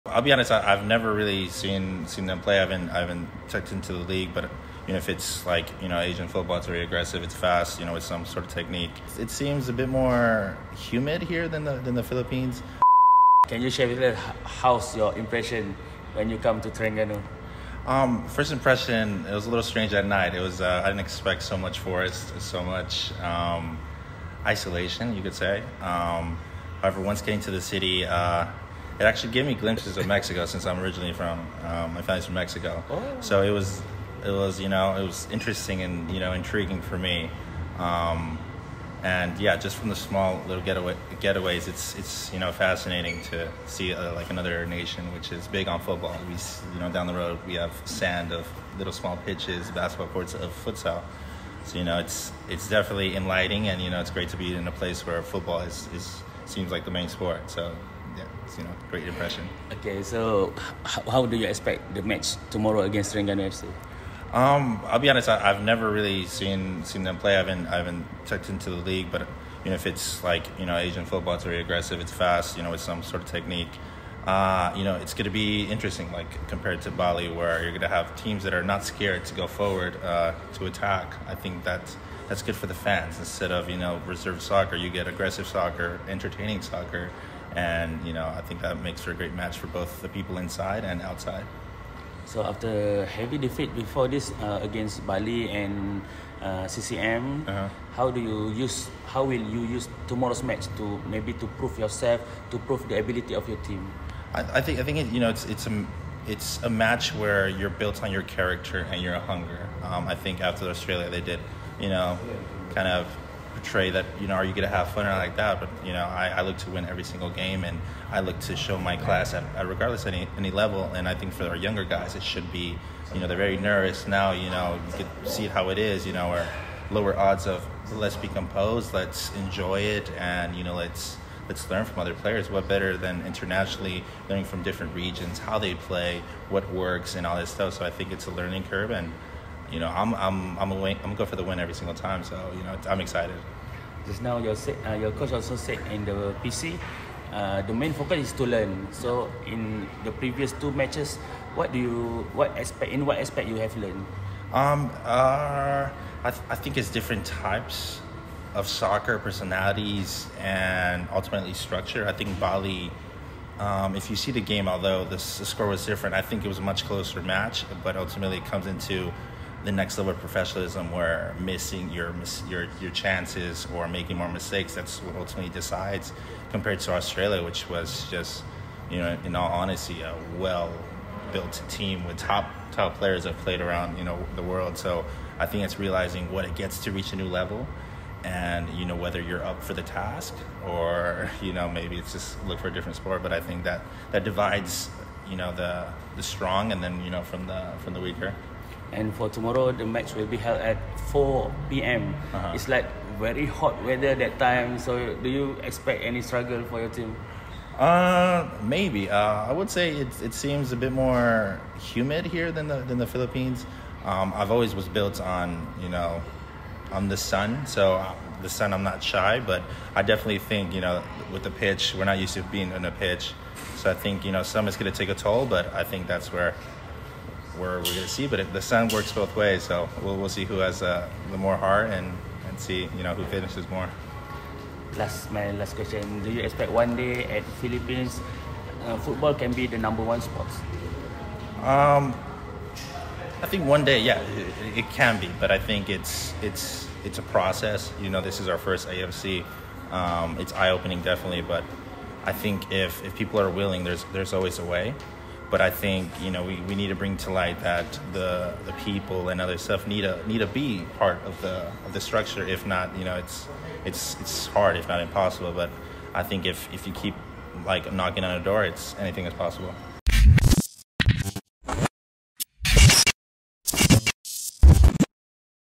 "Jujur saya tak pernah lihat Terengganu FC bermain. Tapi saya tahu bola sepak Asia sangat laju!”
Temubual eksklusif bersama pemain Stallion Laguna FC, Abraham Placito mengenai jangkaan beliau terhadap perlawanan Piala AFC menentang Terengganu FC yang bakal berlangsung di Stadium Sultan Mizan Zainal Abidin esok.